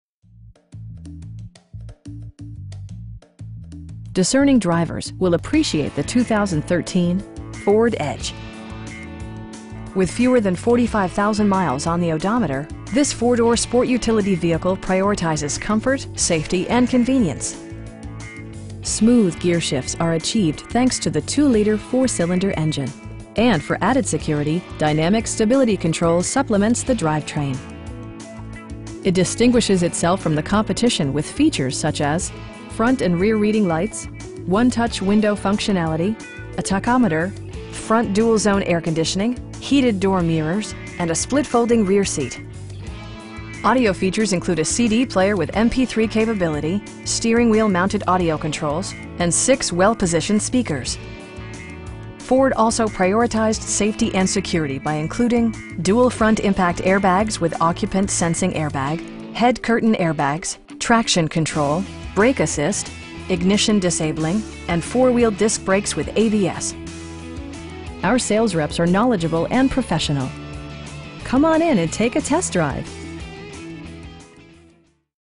2013 Ford Edge SEL in Moline, IL 61265
Sexton Ford
3802 16th Street

Discerning drivers will appreciate the 2013 Ford Edge. With fewer than 45,000 miles on the odometer, this 4 door sport utility vehicle prioritizes comfort, safety and convenience. Smooth gearshifts are achieved thanks to the 2 liter 4 cylinder engine, and for added security, dynamic Stability Control supplements the drivetrain. It distinguishes itself from the competition with features such as: front and rear reading lights, 1-touch window functionality, a tachometer, front bucket seats, front dual-zone air conditioning, heated door mirrors, and a split folding rear seat. Audio features include a CD player with MP3 capability, steering wheel mounted audio controls, and 6 well positioned speakers. Ford also prioritized safety and security by including: dual front impact airbags with occupant sensing airbag, head curtain airbags, traction control, brake assist, ignition disabling, and 4 wheel disc brakes with ABS. Our sales reps are knowledgeable and professional. We'd be happy to answer any questions that you may have. Come on in and take a test drive!